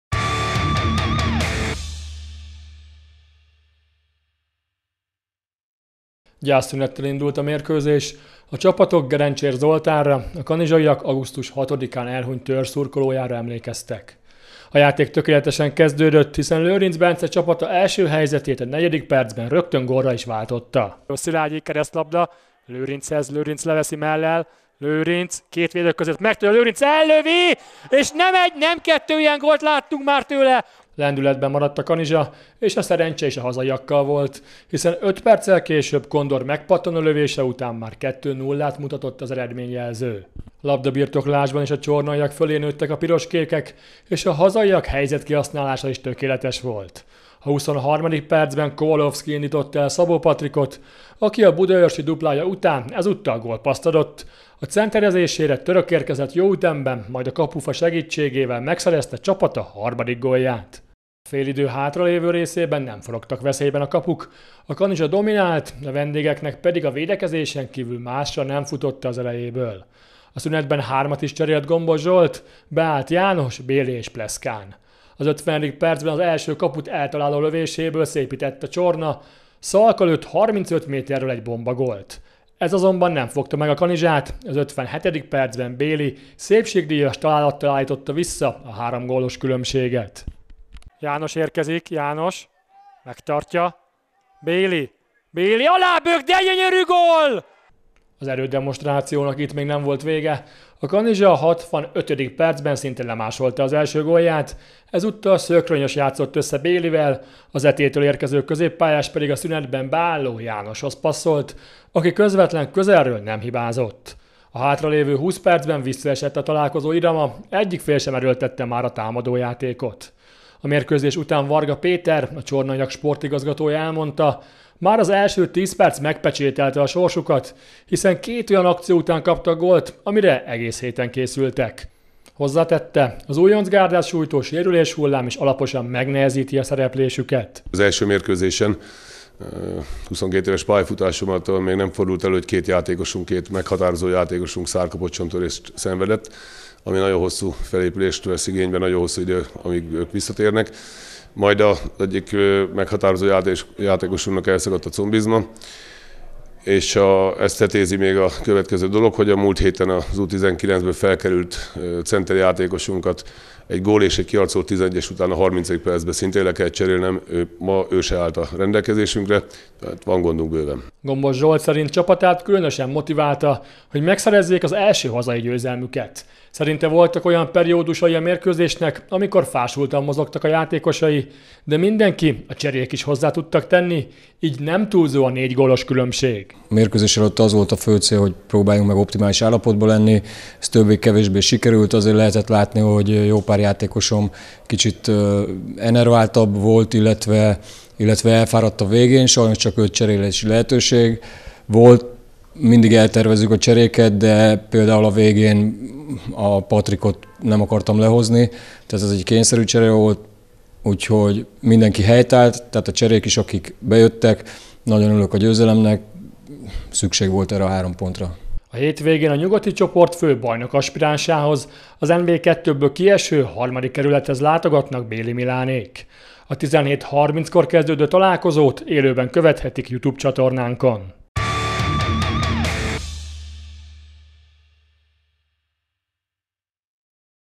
FC Nagykanizsa - Csornai SE összefoglaló
Magabiztosan verte a Csornát az FC Nagykanizsa. Az NB III. hétközi fordulójában Gombos Zsolt csapata két gyors góllal kezdett, így már a találkozó első 10 percben eldőltek az érdemi kérdések. A végeredmény 5-1 lett.